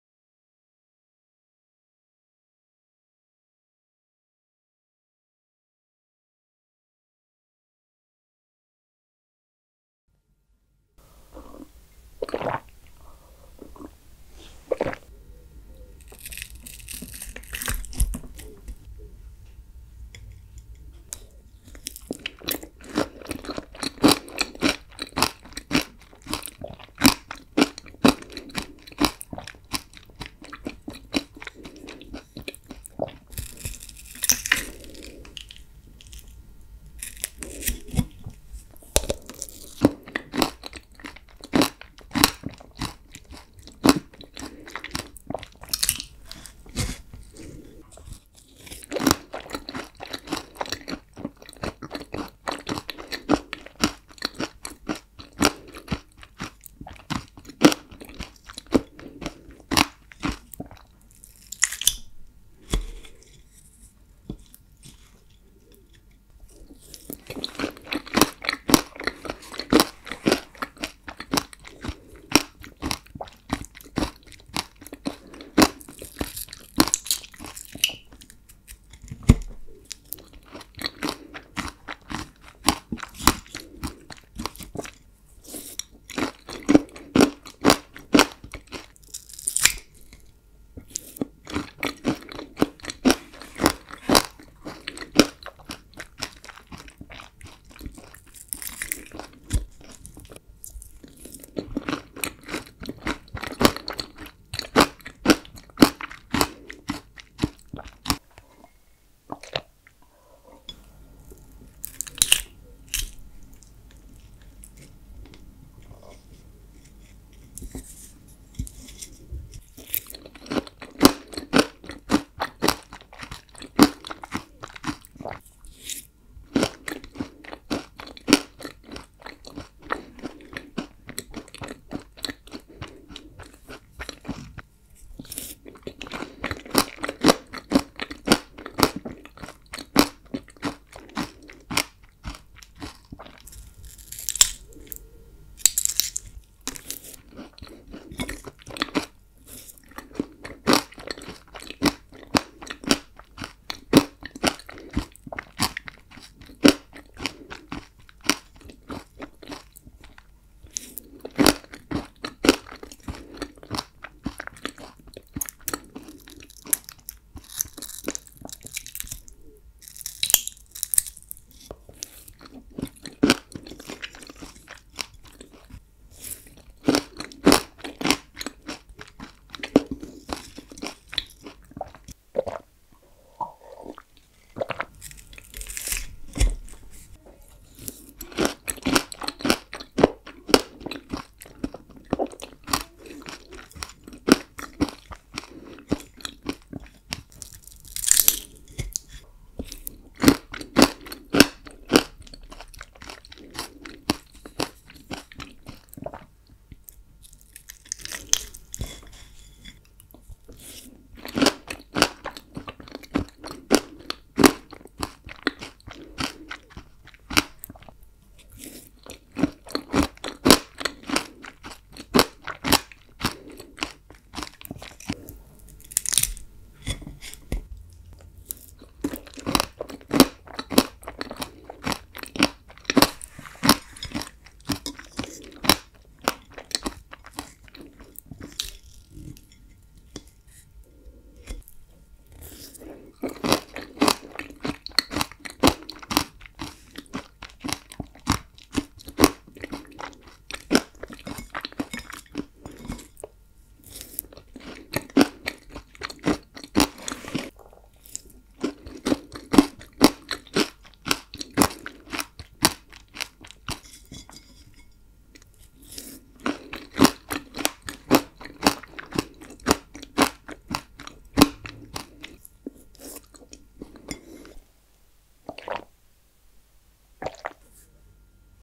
【咀嚼音】ザクロのチーズケーキを食べる【Eating Sounds】
ザクロのチーズケーキを作って食べました。

チーズに合うと聞いてゼリーと二層のチーズケーキに。
チョコケーキと合わせるのもおすすめいただいたので、いつか作りたいです！